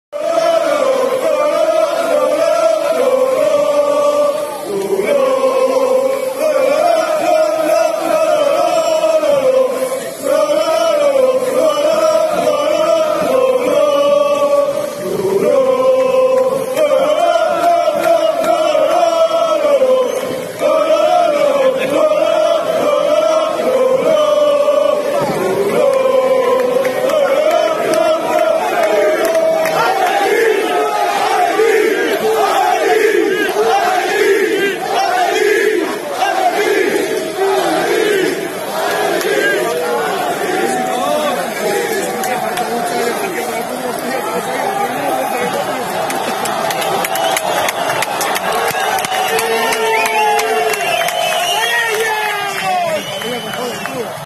Recibimiento de la afición del Athletic club a su equipo
Momentos emocionantes del recibimiento de la afición del Athletic club a su equipo en los aledaños de San Mamés